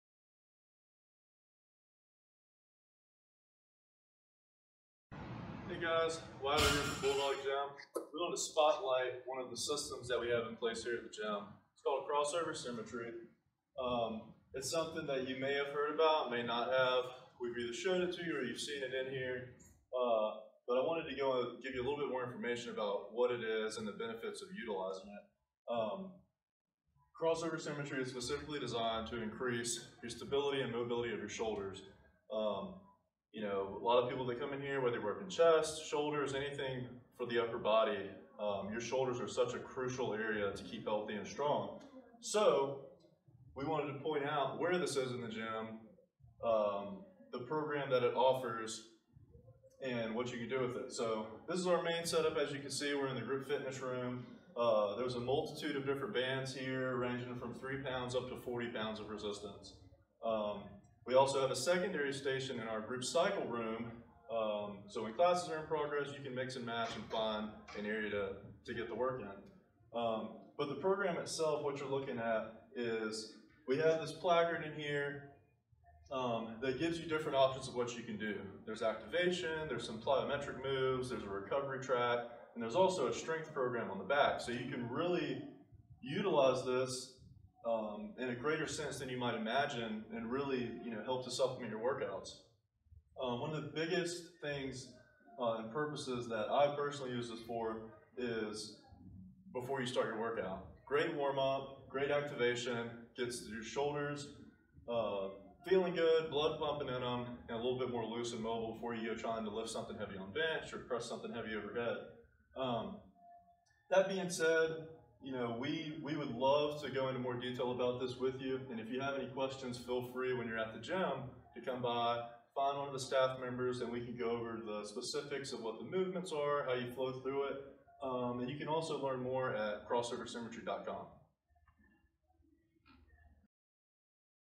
bd101 crossover symmetry
Wyley Deleza gives a high-level overview of the Crossover Symmetry station in this installment of Bulldog 101.